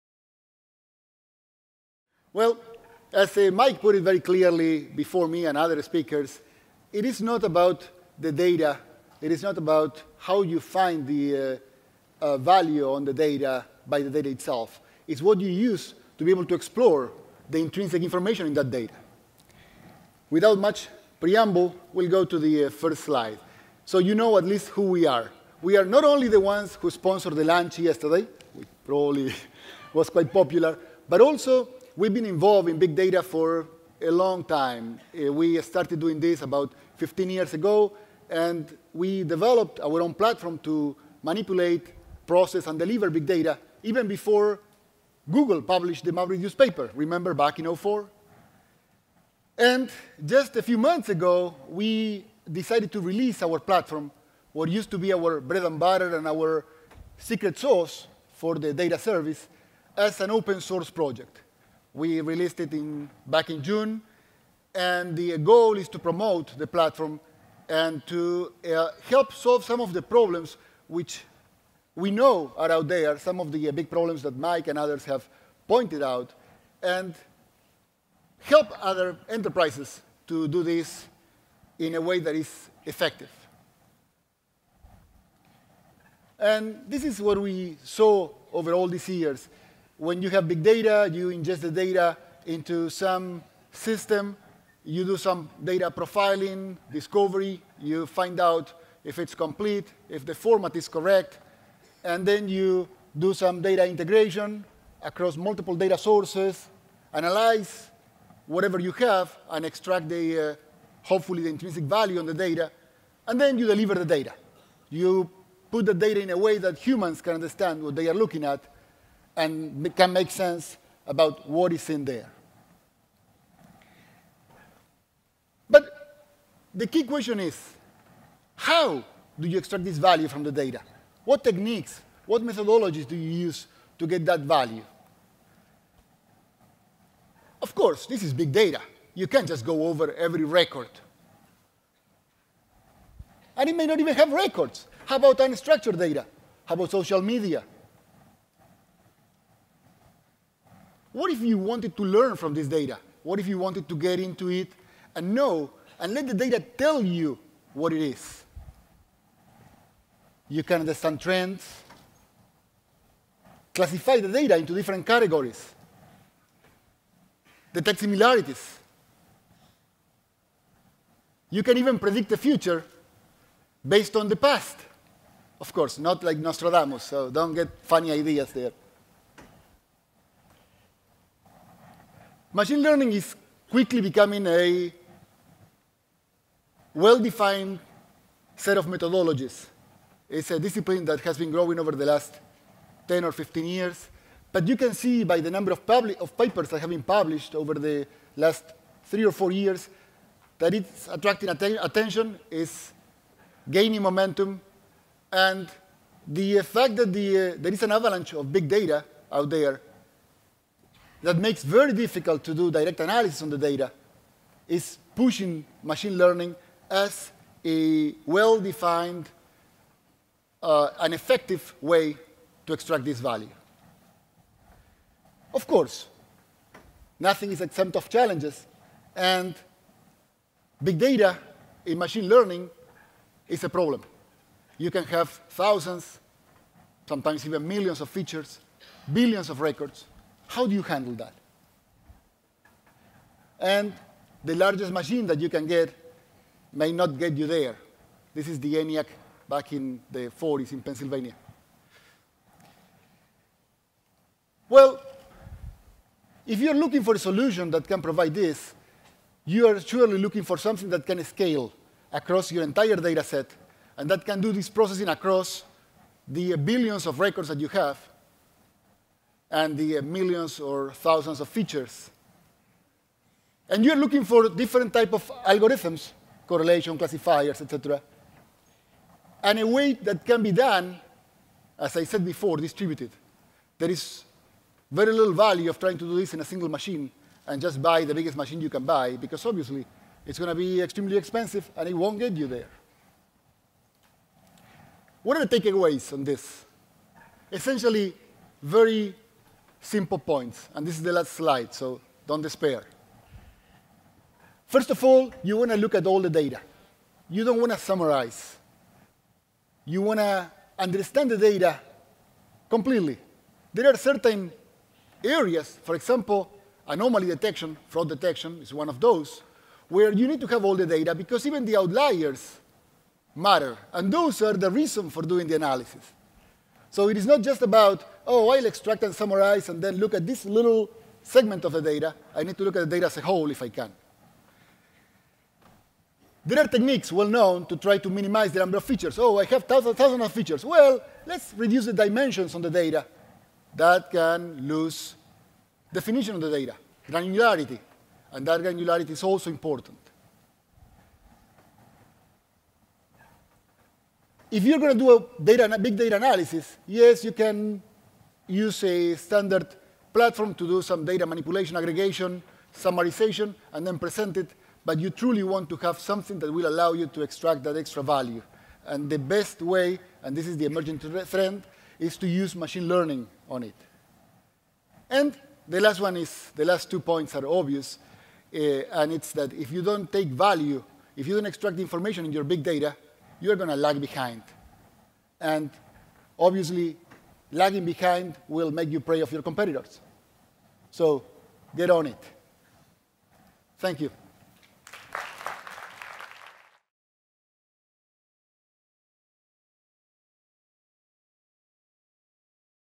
Strata 2012: Flavio Villanustre, "Machine Learning and Big Data: Sustainable Value or Hype?"
Back in the late 80s artificial intelligence was set to take over the world; it didn't happen. In 2012; AI has been stripped down, dressed up and reborn as machine learning. Will it take over the world this time? What makes a Big Data -- Machine Learning solution 'better'? Can machine learning happen with legacy tools? What exactly does it mean to be fully parallel? Do I care? Will I be any better if I get it right?

Flavio Villanustre

LexisNexis Risk Solutions and HPCC Systems

Flavio Villanustre is the Vice President of Infrastructure and Products. In this position, Flavio is responsible for Information and Physical Security, overall infrastructure strategy and new product development for LexisNexis Risk Solutions and HPCC Systems. Prior to 2001, Flavio served in a variety of roles at different companies including Infrastructure, Information Security and Information Technology. In addition to this, Villanustre has been involved with the Opensource community for over 15 years through multiple initiatives. Some of these include founding the first Linux User Group in Buenos Aires (BALUG) in 1994, releasing several pieces of software under different Opensource licenses, and evangelizing Opensource to different audiences through conferences, training and education. Before working in technology, Flavio was a neurosurgeon.